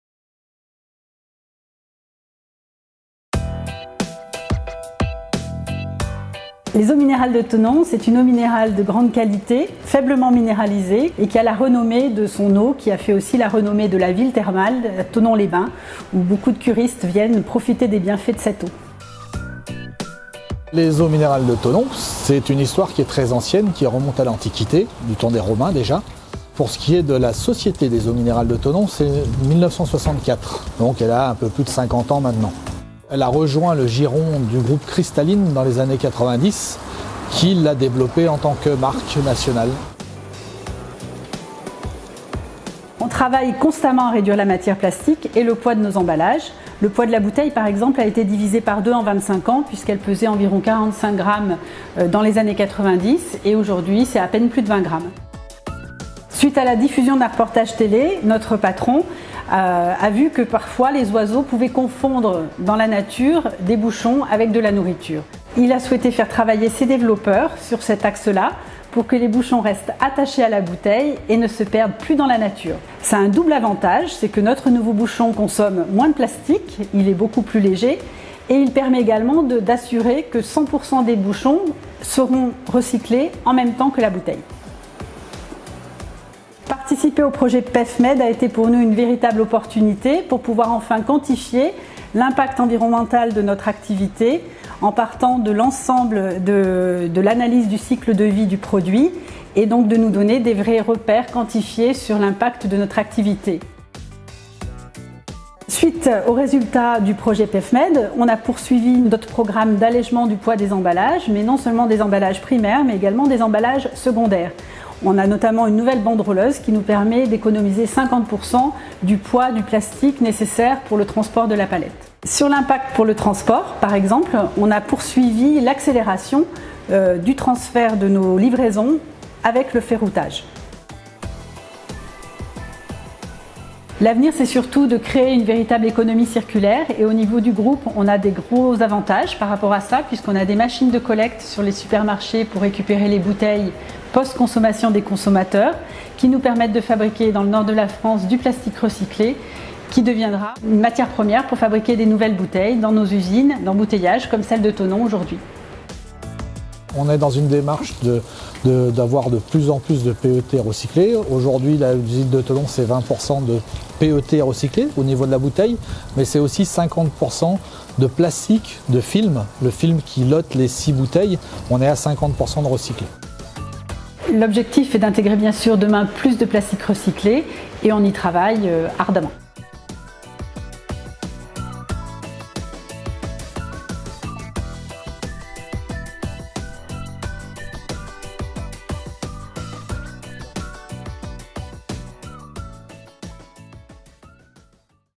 Testimony - Eaux de THONON / PEFMED project (english)
Testimony of a participating company in the PEFMED project, a European project, in witch ANIA is a partner, which aims to assist companies wishing to measure the environmental impact of their products.